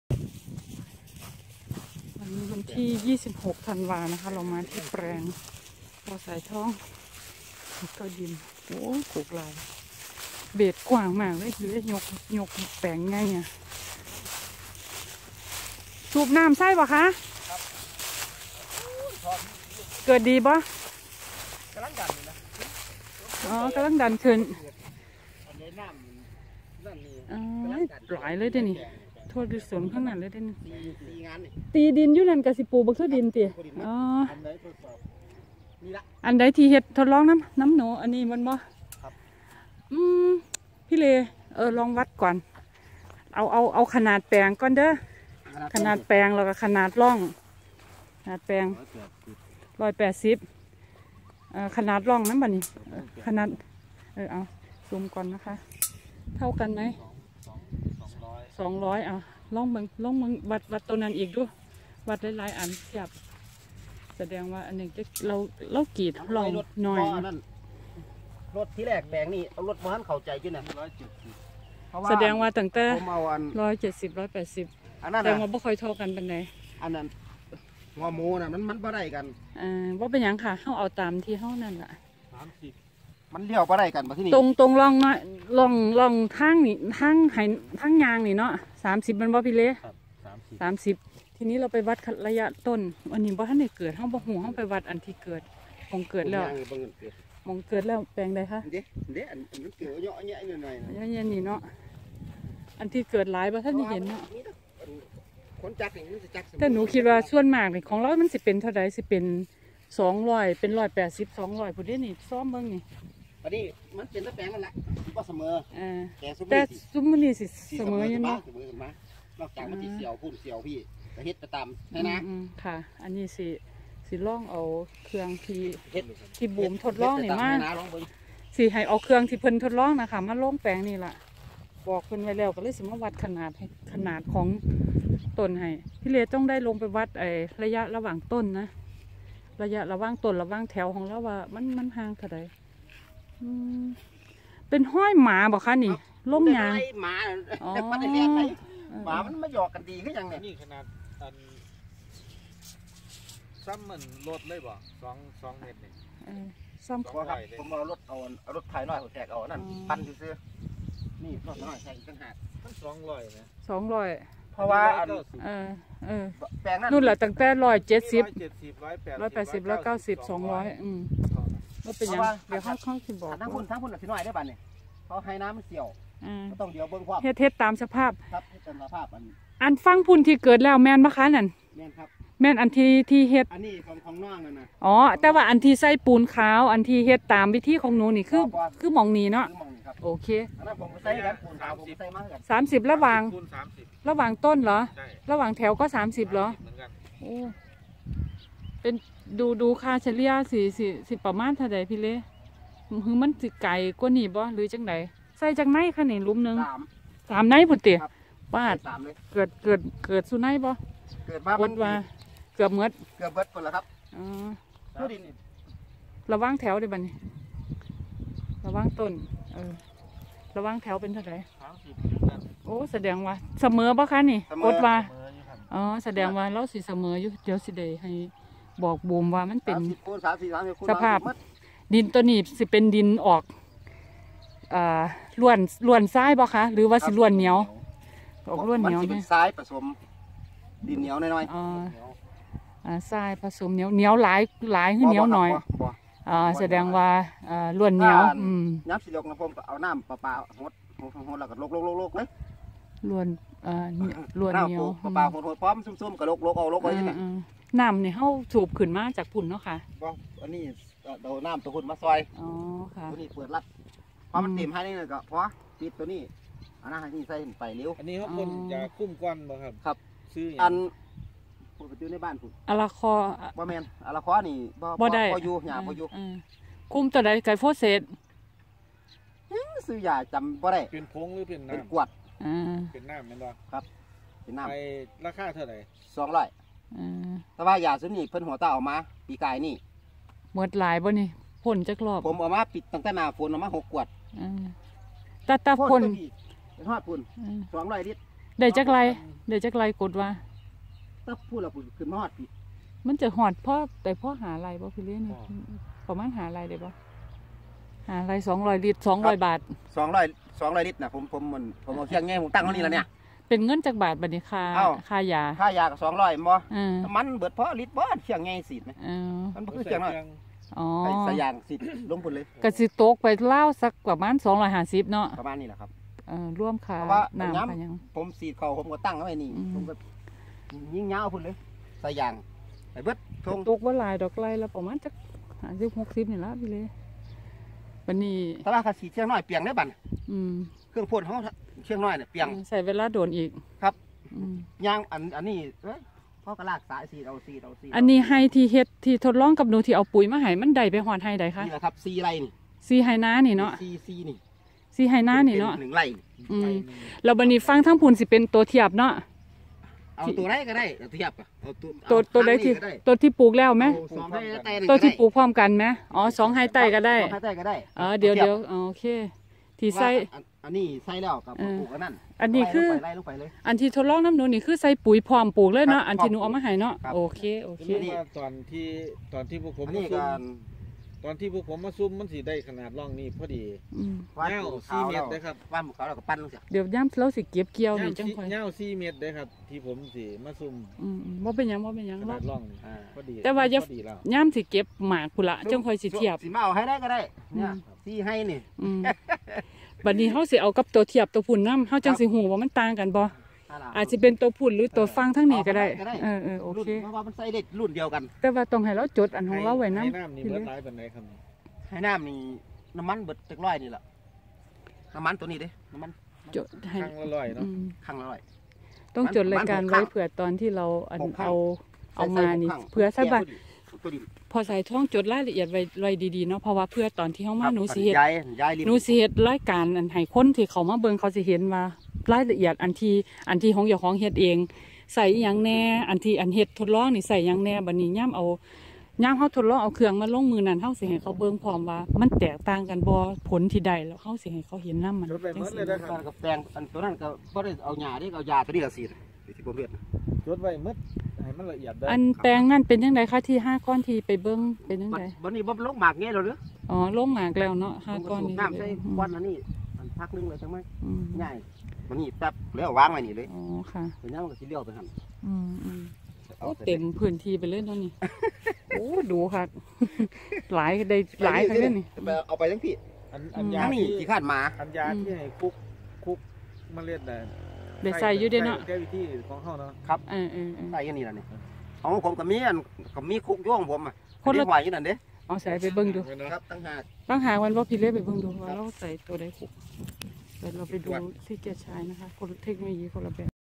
แปลงถั่วลิสงเริ่มงอกแล้ว ดูความงอกและ วัดขนาดแปลง
ลงแปลงถั่วลิสงพ่อสายทอง พื้นที่ปลูกมากกว่า 5 ไร่ วัดระยะปลูก ระยะร่อง ระยะแถว ขนาดแปลง